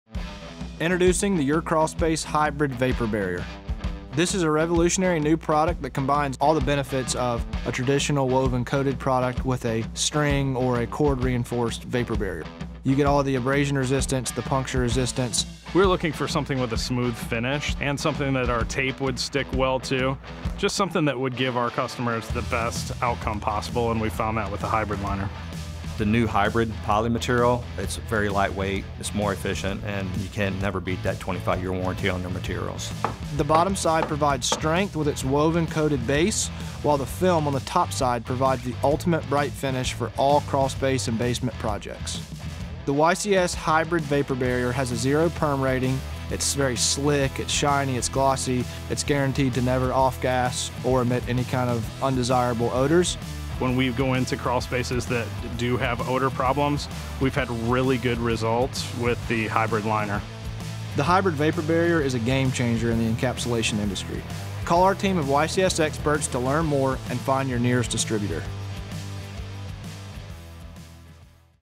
YCS Hybrid Vapor Barrier
The YCS vapor barrier is the newest product released by the YCS Team. Launching in June of 2024, this innovative vapor barrier is a cross between our string and woven products. It is truly a game changer for encapsulation.